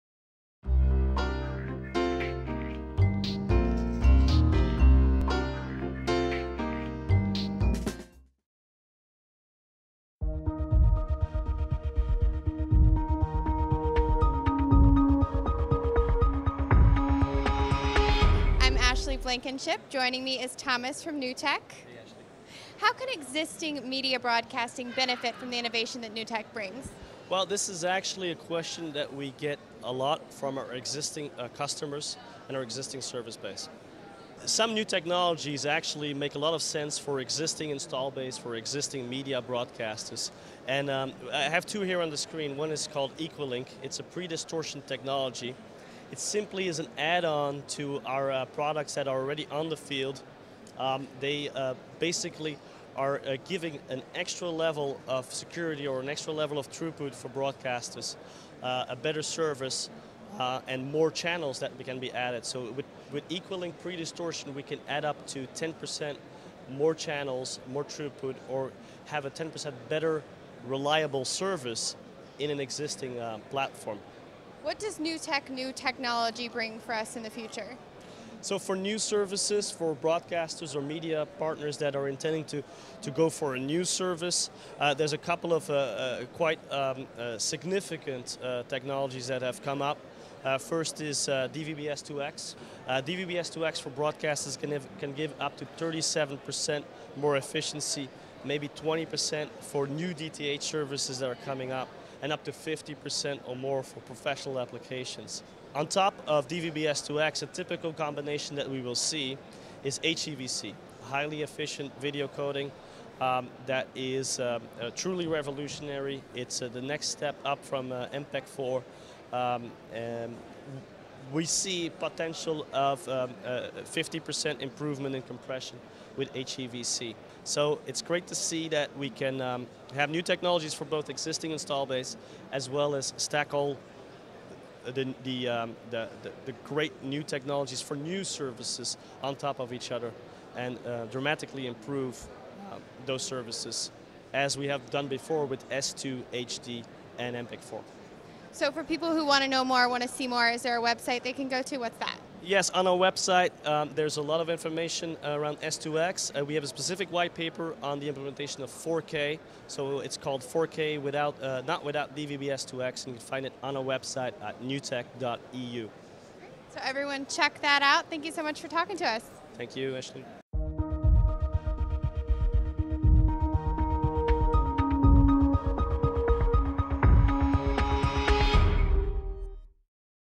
InBroadcast InSight at BroadcastAsia2016 - Newtec Efficiency
InBroadcast’s Ashley Blankenship asks Newtec Chief Commercial Officer Thomas Van den Driessche about just how media broadcasting can benefit from the latest innovations from Newtec…